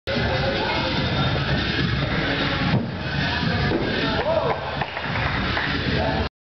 Roff BF Doublefull attempt
roundoff backflip doublefull versuch...
mitlerweile klappts schon besser als aufm video aber noch nich ganz ^^